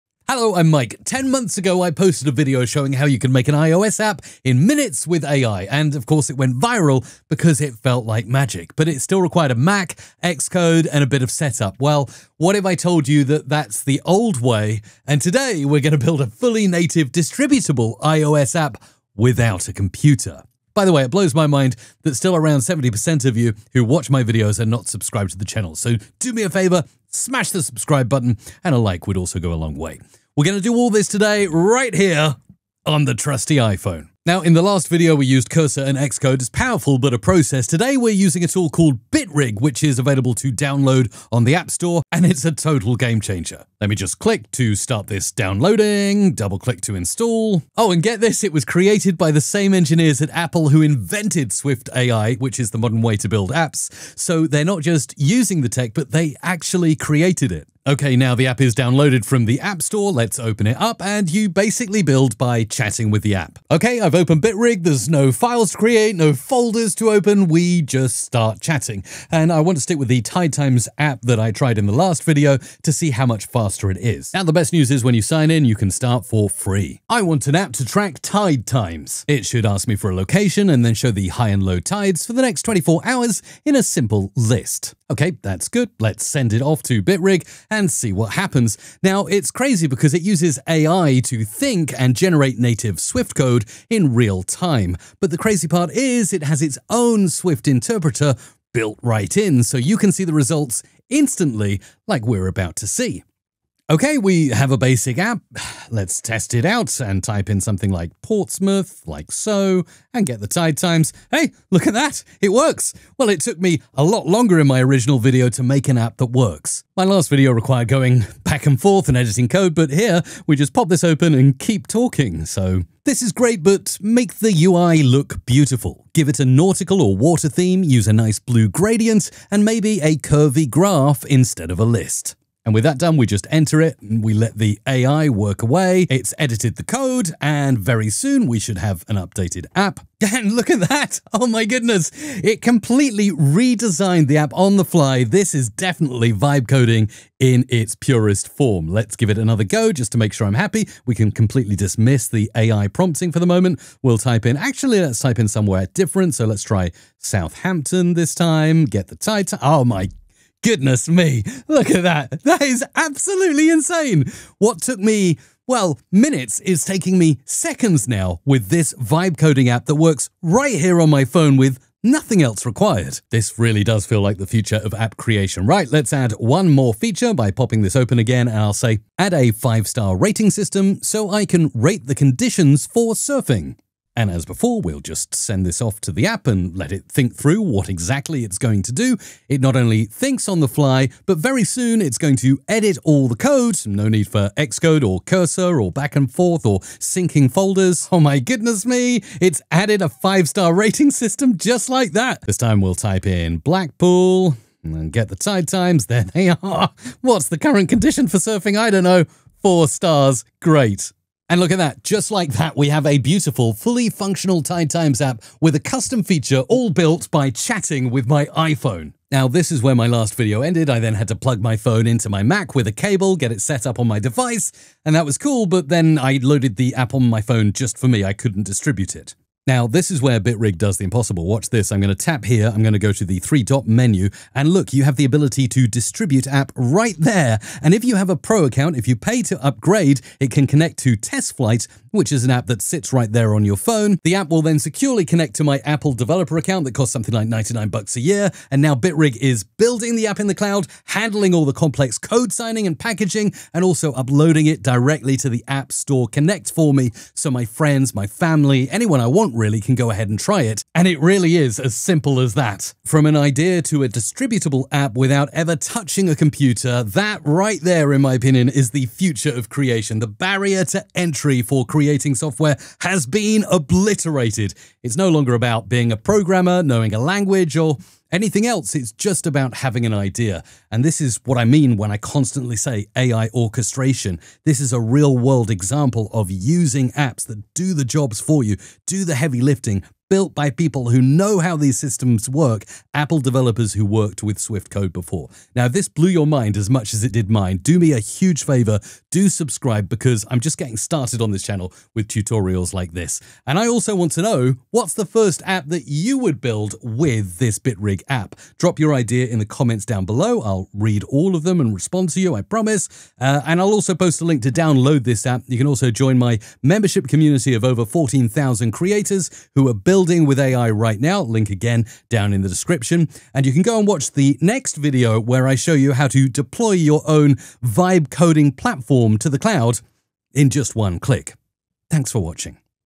I Built an iOS App ON MY PHONE!
Remember when I made an iOS app with AI and it went viral? Well, that method is officially obsolete. In this video, I'm showing you the future, and it's even crazier... I build a fully native, beautiful, and distributable iOS app from scratch without ever touching a computer. Using a mind blowing new tool called bitrig, I go from a simple text prompt to a working Tide Times app in seconds, adding features and redesigning the entire UI just by chatting with AI, all on my iPhone. The real magic happens when I skip the computer entirely and can publish the app directly to TestFlight, ready for anyone to try.

00:00 The Old Way is Obsolete
00:34 Meet bitrig: The App Store Game-Changer
01:17 My First Prompt: Building the App
01:43 Instant Results: The App is ALIVE!
02:02 Vibe Coding a Beautiful UI
02:59 Adding a New Feature By Chatting
03:49 The Impossible Step: Publishing to TestFlight
04:22 The Future of Creation is Here
05:03 What App Would YOU Build?